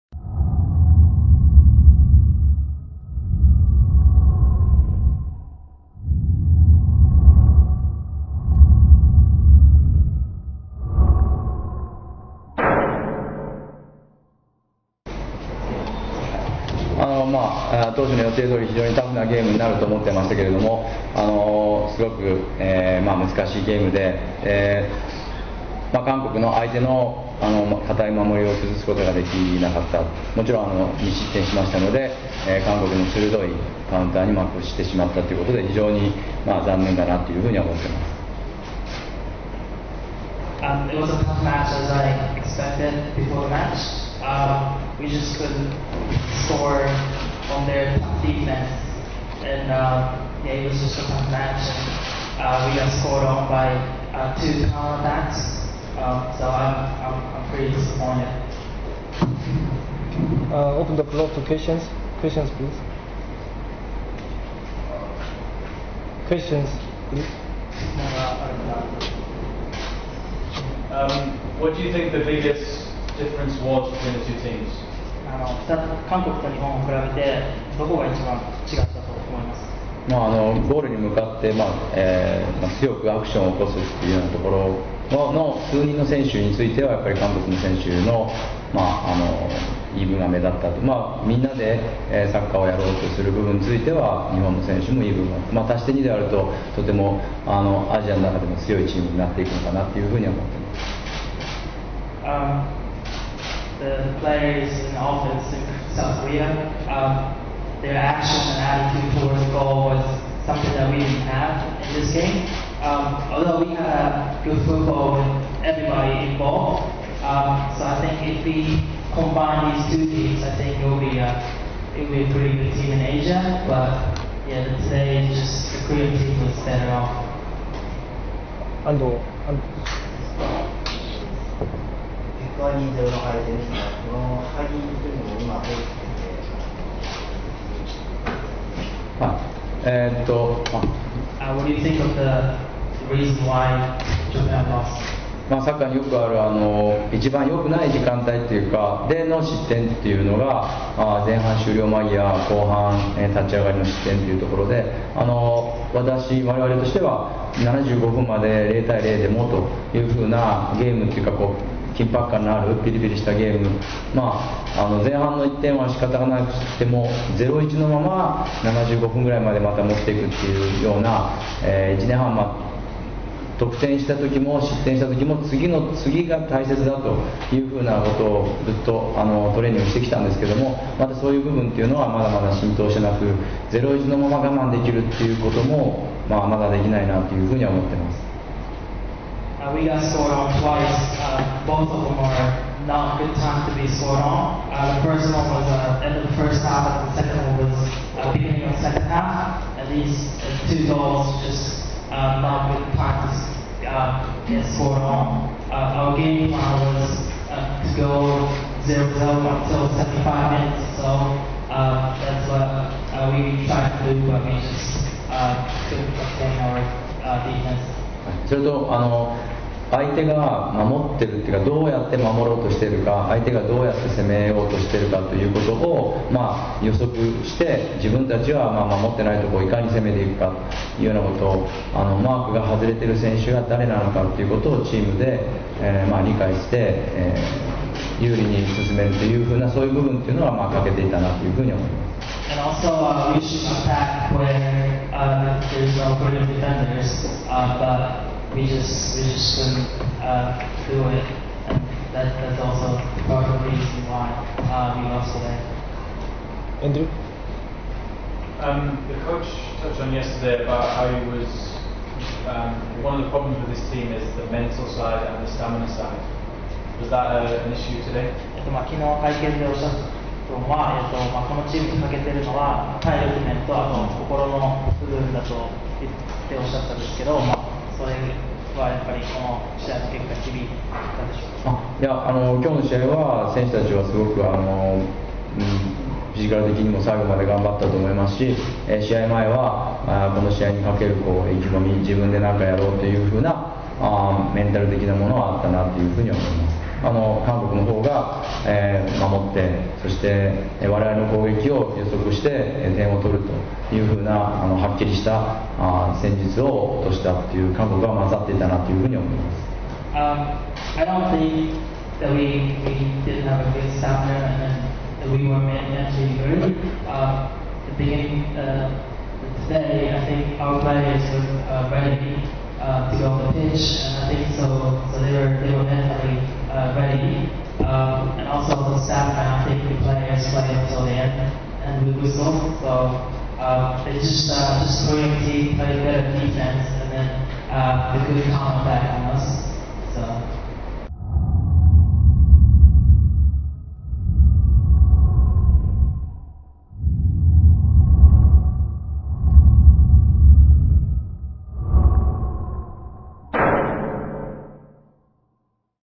Hirofumi Yoshitake Press Conference after game lose Korea Republic 0-2
เลขาเดย์  @ sps th